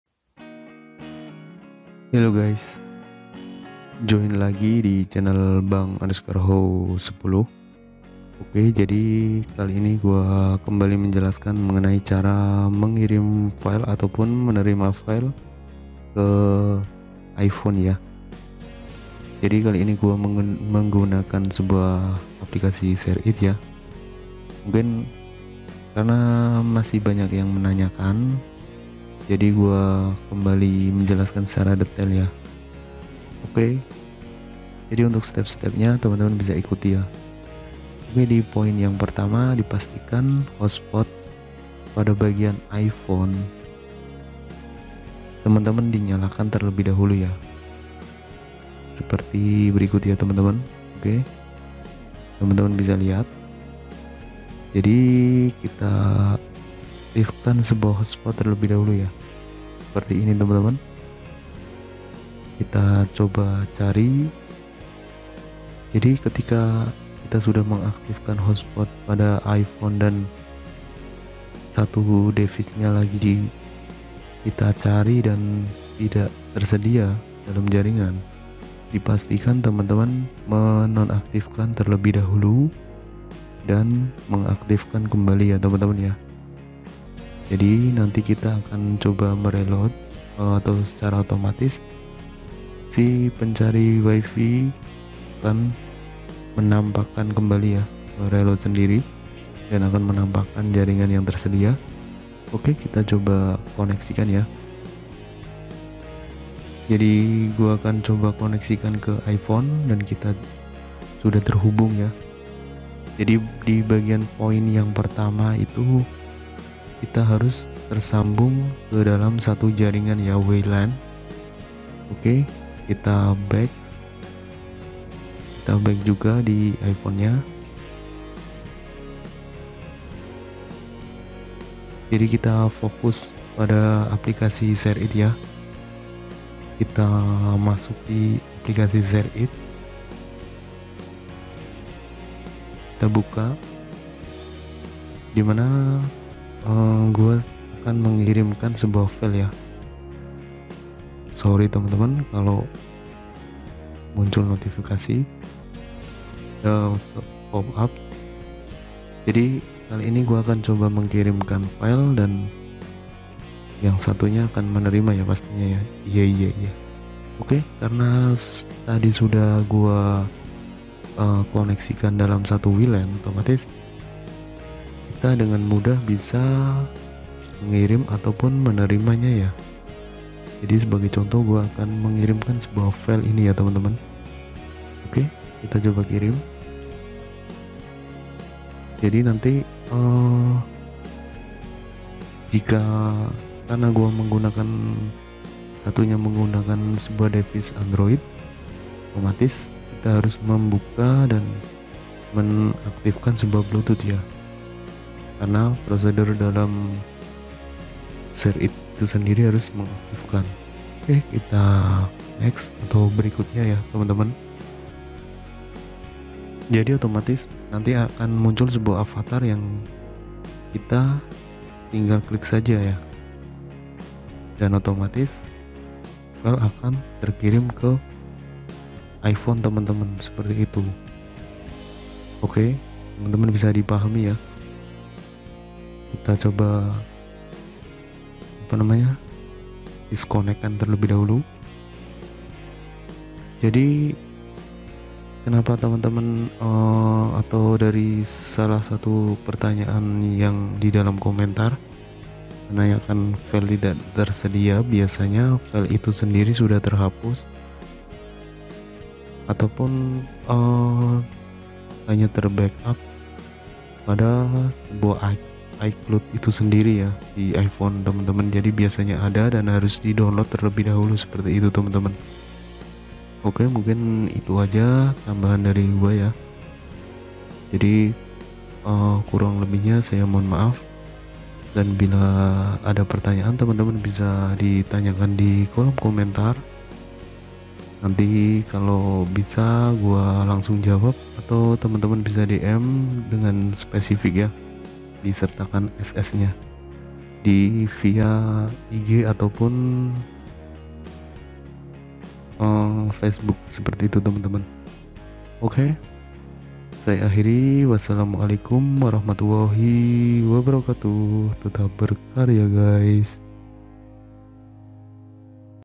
Cara Mengirim Atau Menerima File Pada iPhone (100% Worked)
Video kali ini adalah sebuah cara mengirim ataupun menerima file pada iPhone.
Jadi bagi temen-temen yang kesusahan saat mengirim, banyak kendala baik dari diavatar tidak ditemukan, file tidak tersedia dan lainya.
Silahkan simak saja video ini yang saya jelaskan didalamnya.
Semoga bisa bermanfaat bagi temen-temen yang membutuhkanya.
Jika adapertanyaan bisa tanyakan saja dikolom komentar ya guys.
Terimakasih.